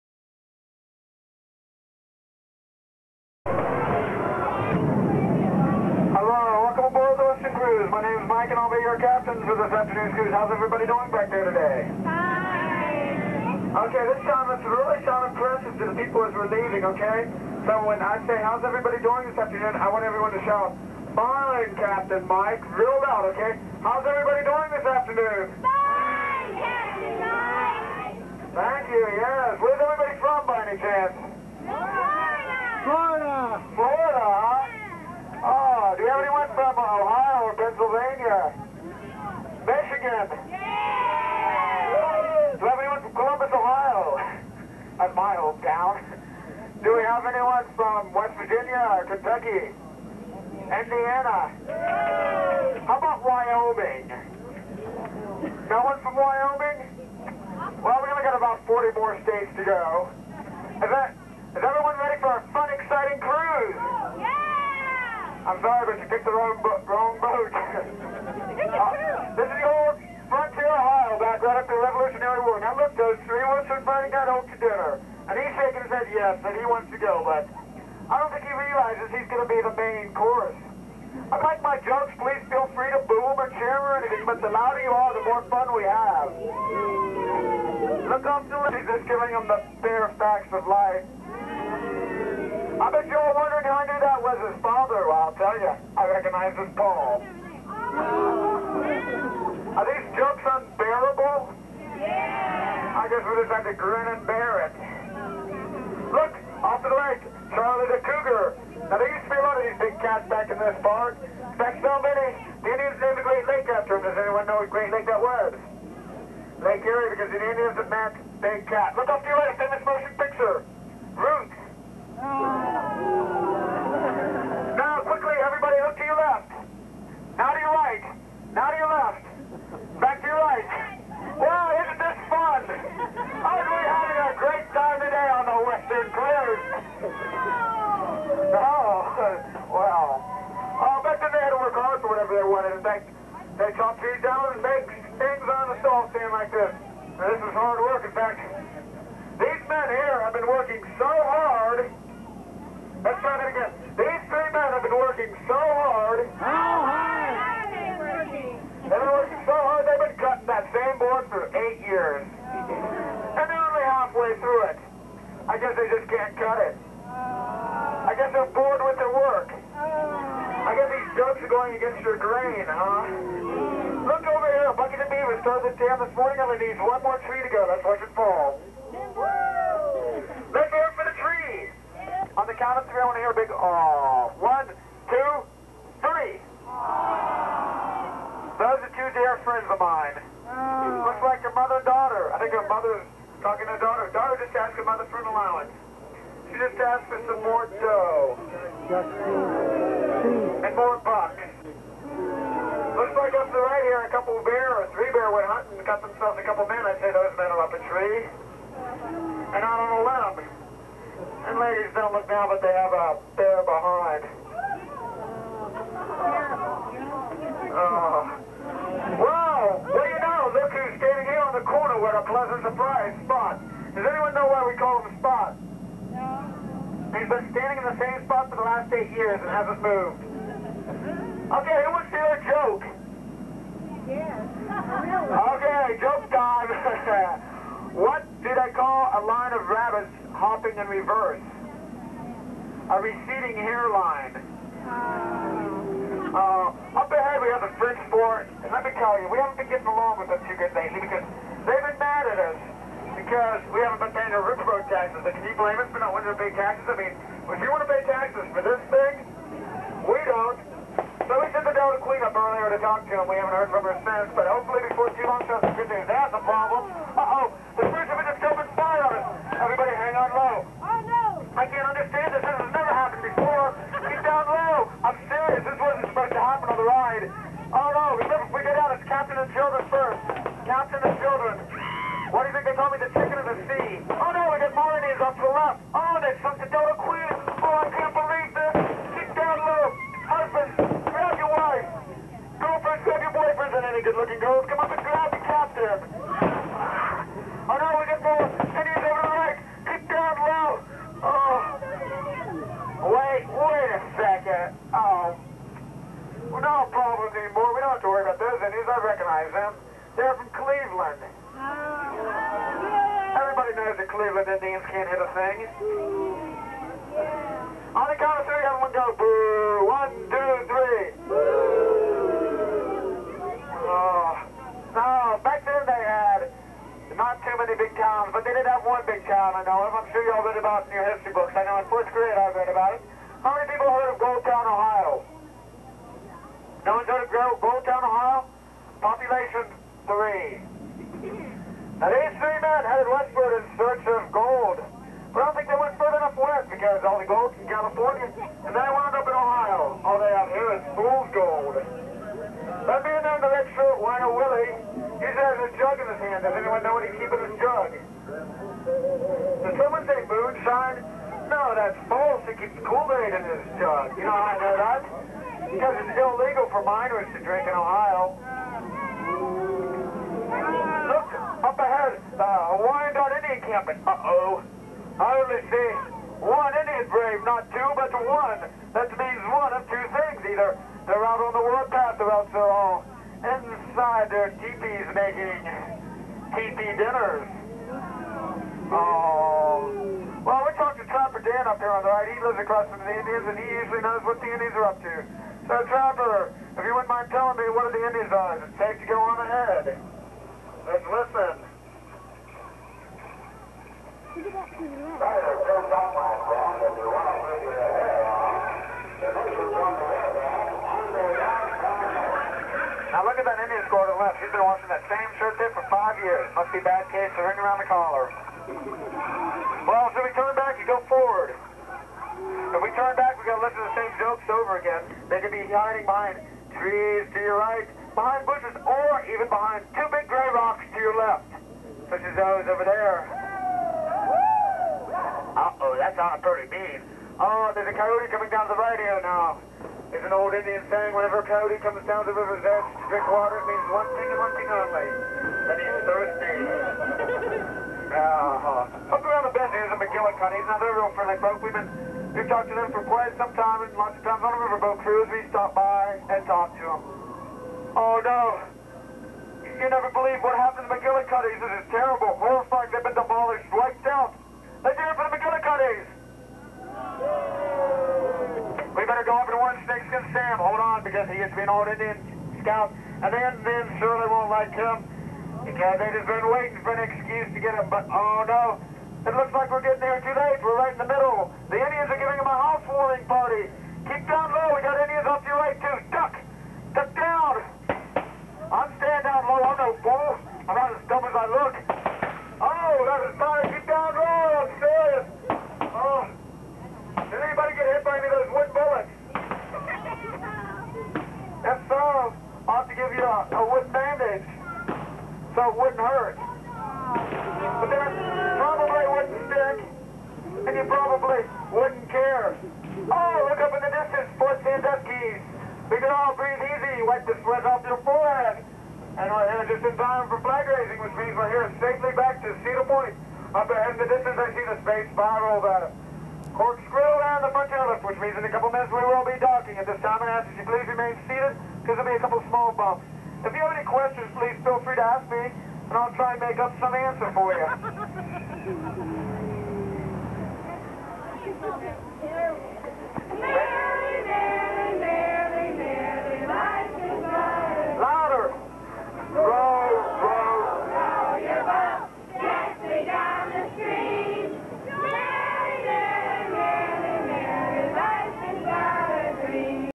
Cedar Point - Riverboat (Paddlewheel) Ride -1980
This boat ride at Cedar Point amusement park in Sandusky, Ohio, transported guests on a relaxing trip around Cedar Point's lagoon.  It was originally known as "Western Cruise" but was renamed "Paddlewheel Excursions" in 1986.  Shore scenes depicting early Americana.  The ride's best feature was the offbeat humor of its boat captains.  Closed on Labor Day 2011 to make room for 'Dinosaurs Alive'

Our family lugged around one of the original video cameras that connected to the full-size "portable" VCRs of the day.